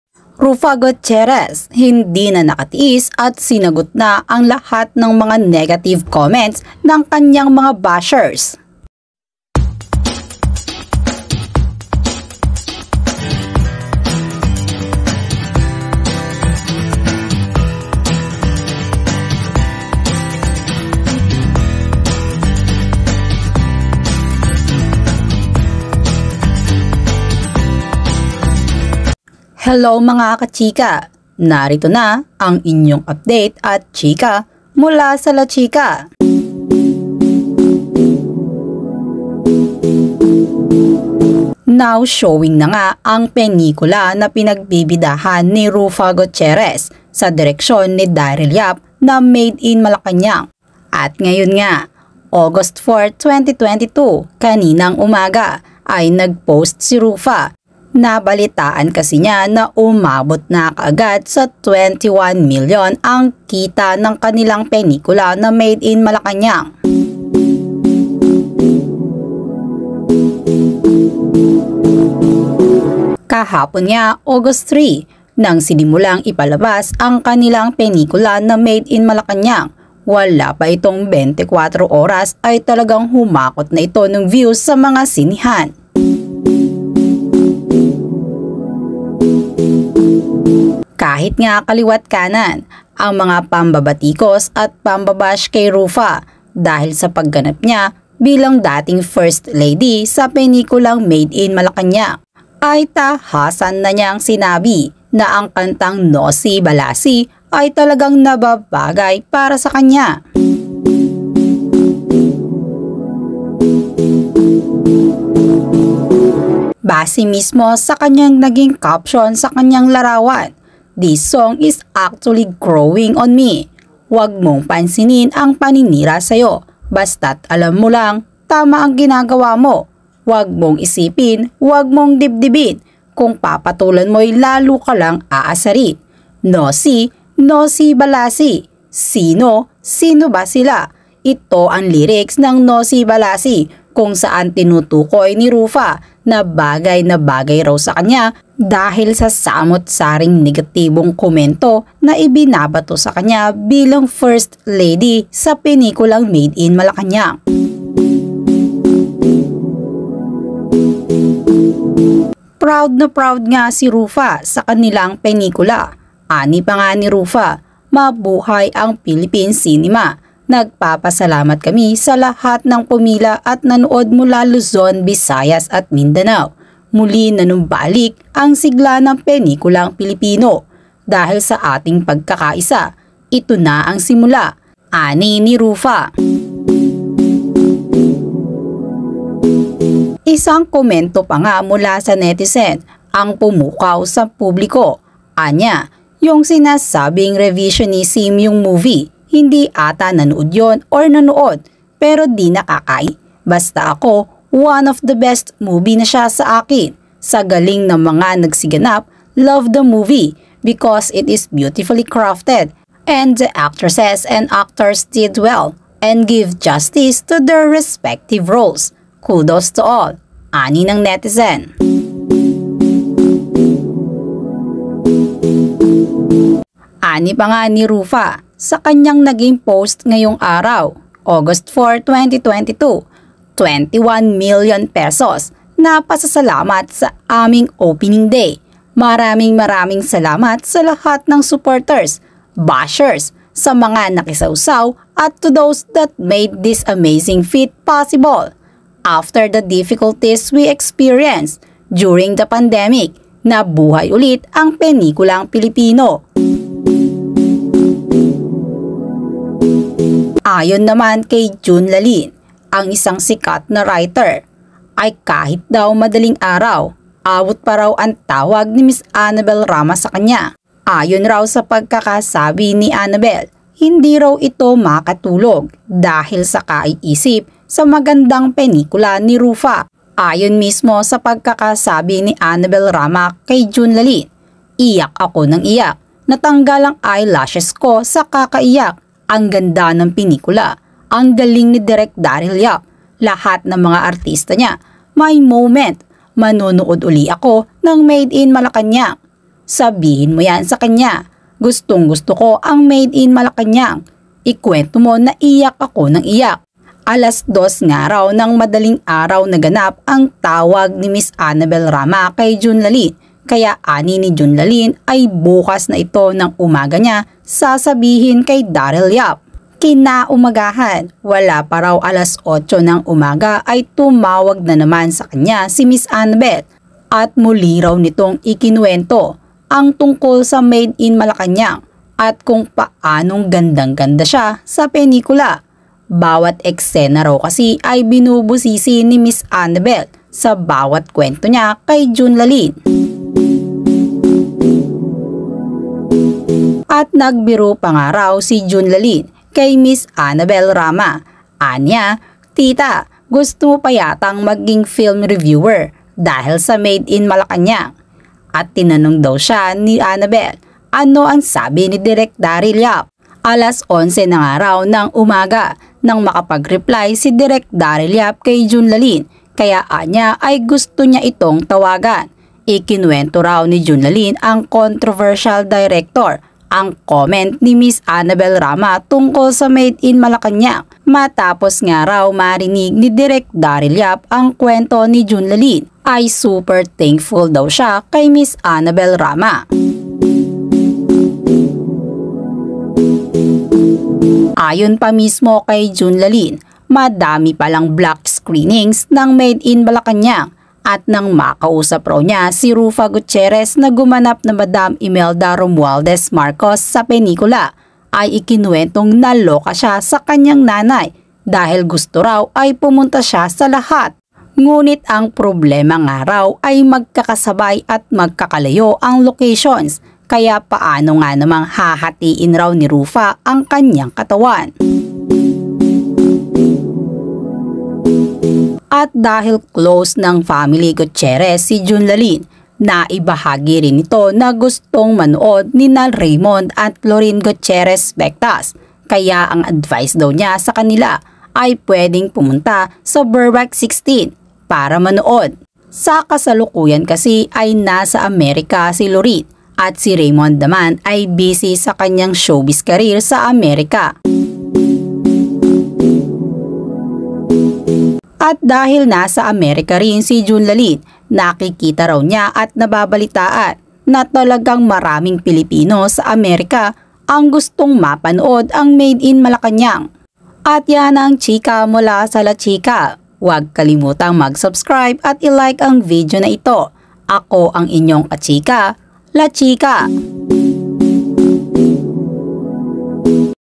RUFFA GIGIL AT HINDI NA NAKATIIS, SINAGOT NA ANG KANYANG MGA BASHERS NG "SINO BA SILA?!"
RUFFA GUTIERREZ HINDI NA NAKATIIS AT SINAGOT NA ANG KANYANG MGA BASHERS NG "SINO BA SILA?!"